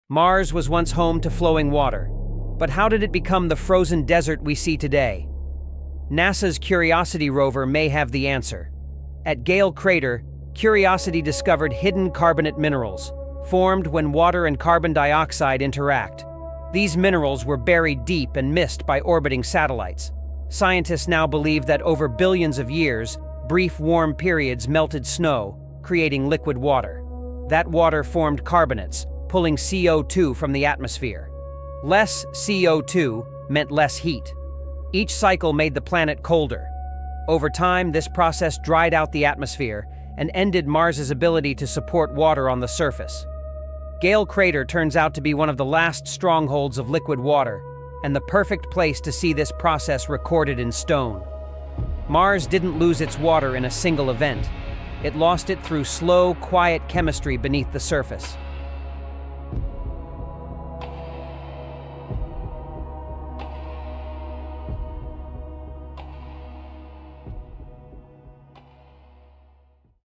MARS: DRIED OUT FROM WITHIN?
A new study points to meltwater-triggered carbonate formation as a possible reason Mars lost its ability to support water on the surface.

These deposits, found by Curiosity at Gale Crater, may have pulled CO2 from the air slowly enough to change the planet forever.